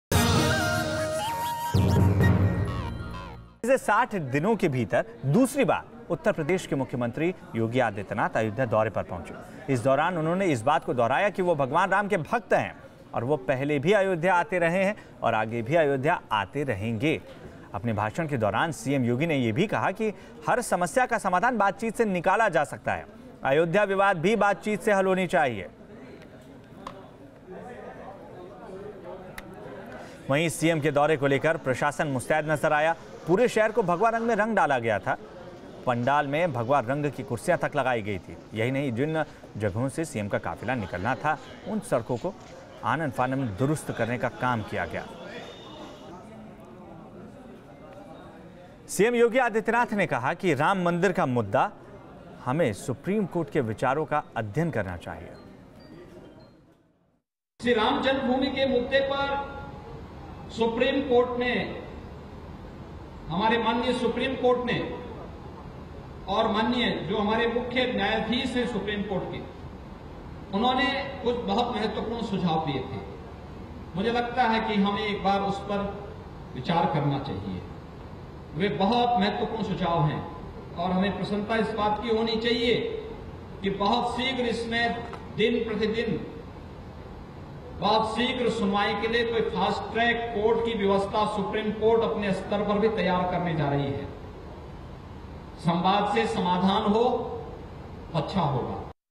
UP CM Yogi Adityanath visit to Ayodhya
Uttar Pradesh Chief Minister Yogi Adityanath has identified himself as a disciple of Lord Ram first. While speaking on the issue of Ram Temple at Ayodhya, he asserted that he will keep on visiting Ayodhya.